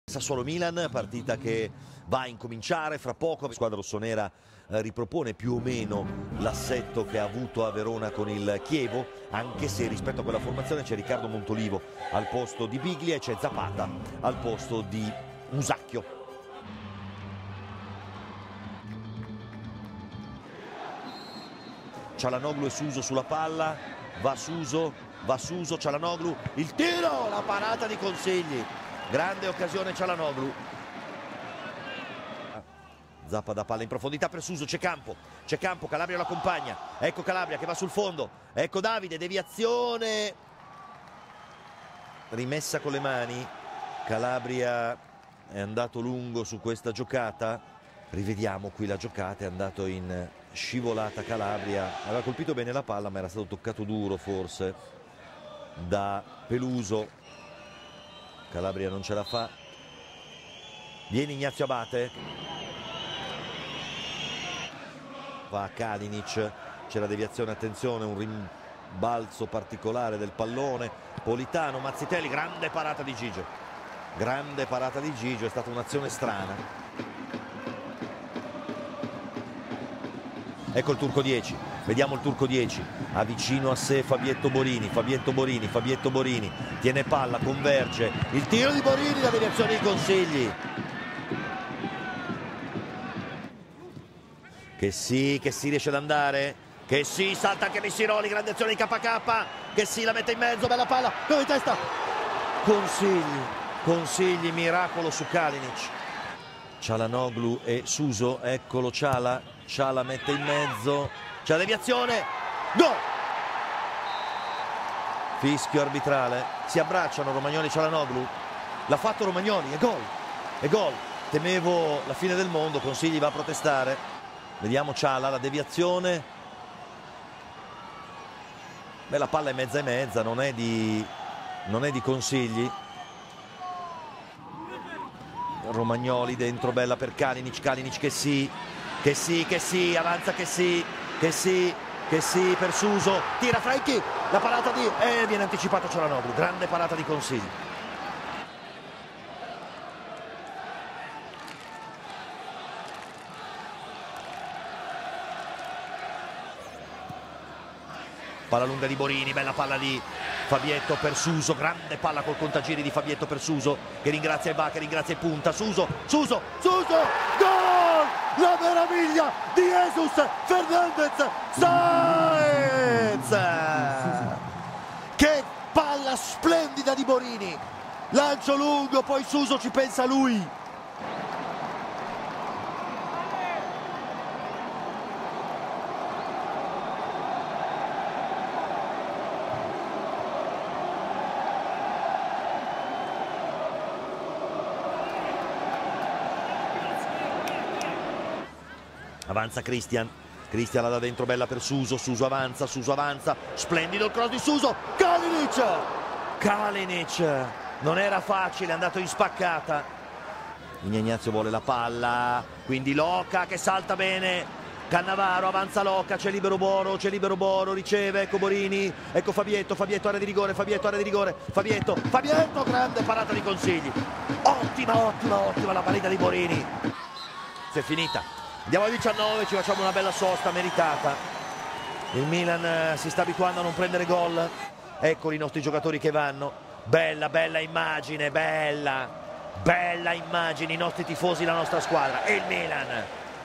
Romagnoli-Suso and AC Milan hit Sassuolo for two
A goal in each half for the Rossoneri who win their second consecutive game away from San Siro